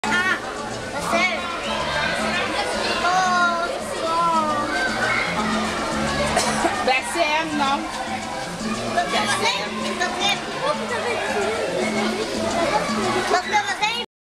Papillon grand porte-queue (machaon), jardin botanique de Montréal, papillons en liberté